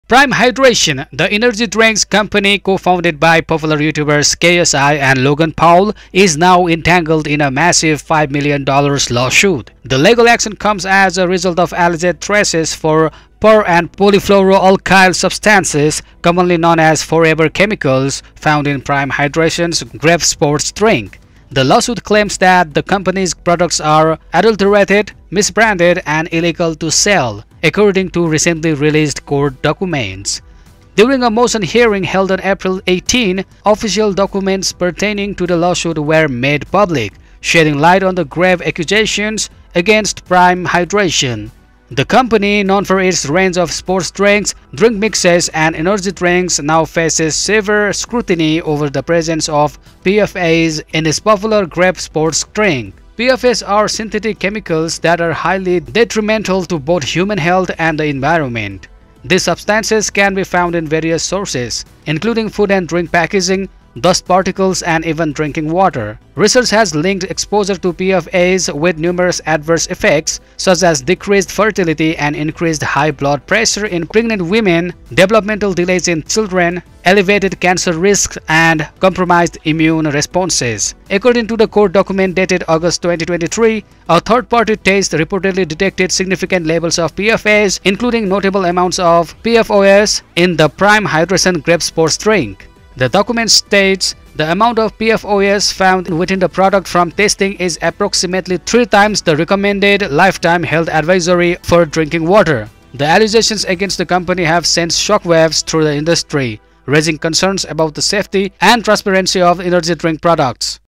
Explosive $5M Lawsuit: Prime Drinks Allegedly Contaminated with 'Forever Chemicals,'
Prime Hydration, the energy drinks company co-founded by popular YouTubers KSI and Logan Paul, is now entangled in a massive $5 million lawsuit. The legal action comes as a result of alleged traces of per- and polyfluoroalkyl substances (PFAs), commonly known as 'forever chemicals,' found in Prime Hydration's Grape Sports Drink. The lawsuit claims that the company's products are "adulterated, misbranded, and illegal to sell," according to recently released court documents.
During a motion hearing held on April 18, official documents pertaining to the lawsuit were made public, shedding light on the grave accusations against Prime Hydration. The company, known for its range of sports drinks, drink mixes, and energy drinks, now faces severe scrutiny over the presence of PFAs in its popular Grape Sports Drink.
PFAs are synthetic chemicals that are highly detrimental to both human health and the environment. These substances can be found in various sources, including food and drink packaging, dust particles, and even drinking water. Research has linked exposure to PFAs with numerous adverse effects, such as decreased fertility and increased high blood pressure in pregnant women, developmental delays in children, elevated cancer risks, and compromised immune responses.
According to a court document dated August 2023, a third-party test reportedly detected significant levels of PFAs, including notable amounts of perfluorooctanesulfonic acid (PFOS), in the Prime Hydration Grape Sports Drink. The document states, "The amount of PFOS found within the product from testing is approximately three times the recommended lifetime health advisory for drinking water."
The allegations against the company have sent shockwaves through the industry, raising concerns about the safety and transparency of energy drink products.